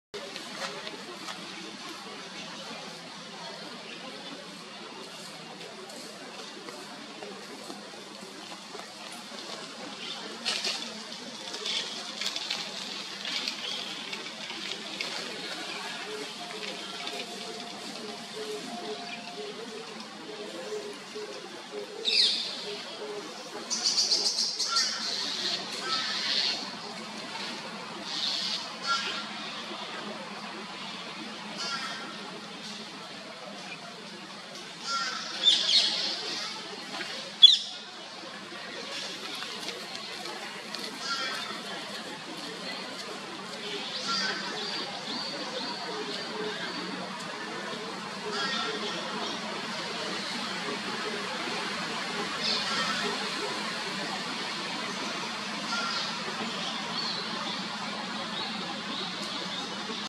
ツミの雛への給餌 - Japanese sparrowhawk -
まだ小さな雛がちらりと見えました。